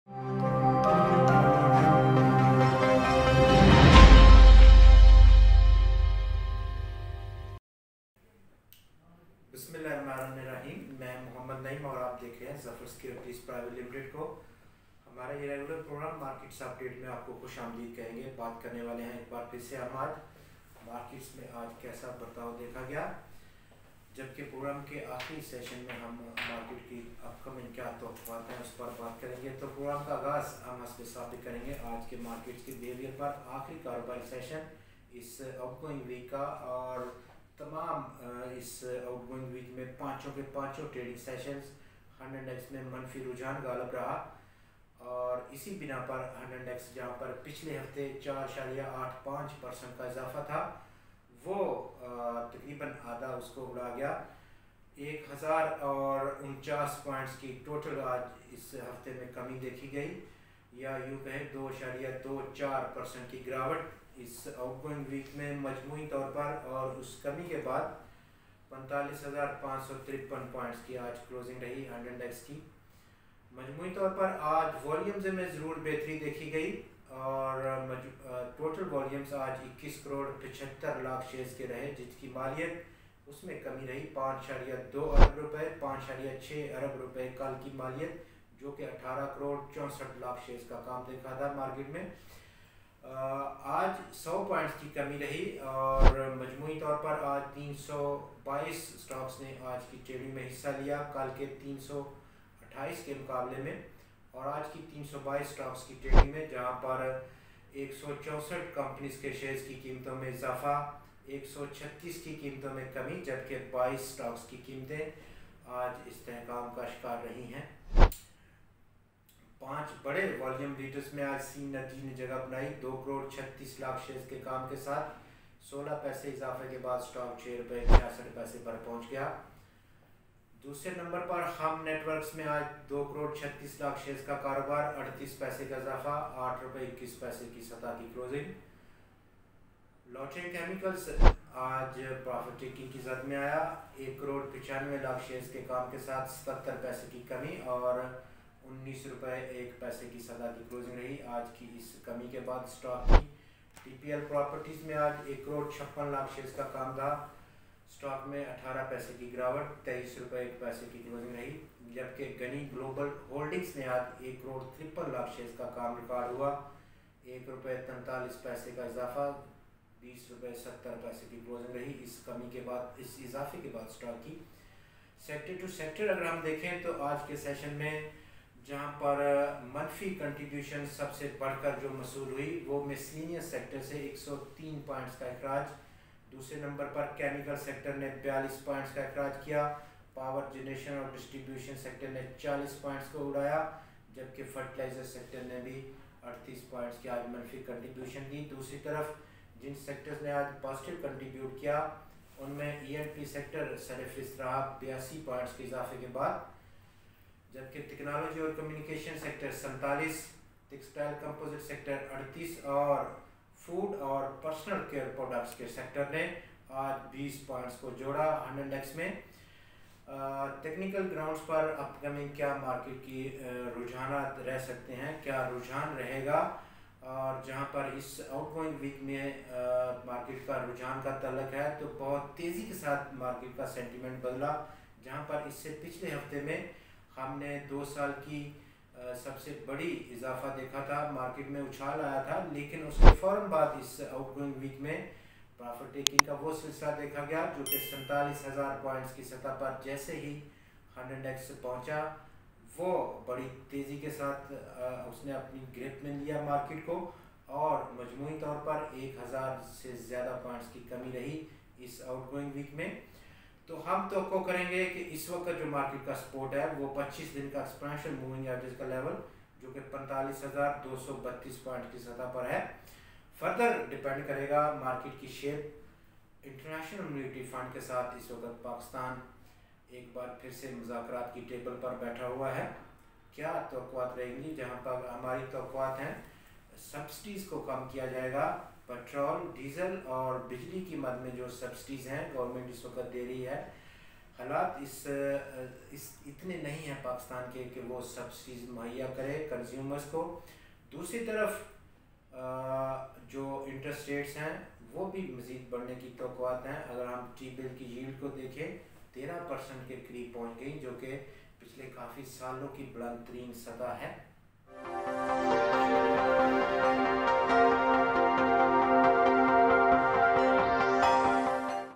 PSX Pakistan Stock Exchange Today Market Update (in Urdu) 22 April 2022
PSX Pakistan Stock Exchange Today Market Update (in Urdu) 22 April 2022 with Naeem Akhtar, Market Analyst, Zafar Securities Pvt. Limited This video explains: - KSE Market Summary - Pakistan Stock Exchange Today - KSE100 index Today - KSE100 index - 100 index - PSX Summary

After gaining almost 4.85% in the previous week the 100 index fell by 1,049 points in the out going week